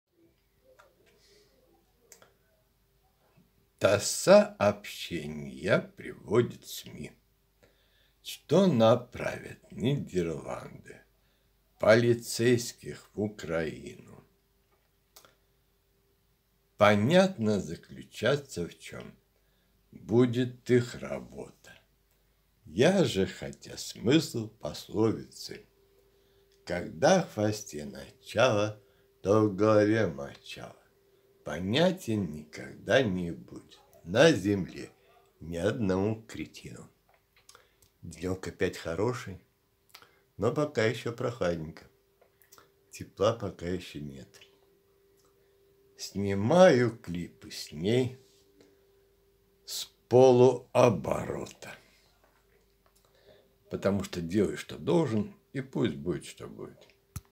Направят Нидерланды полицейских в УКраину
ТАСС сообщения приводит СМИ, что направят Нидерланды полицейских в Украину.
И понятно, заключаться в чем будет их работа.
Я же, хотя смысл пословицы — «Когда в хвосте начало, то в голове мочало» — понятен никогда не будет на Земле ни одному кретину,
Снимаю клипы с ней с полуоборота.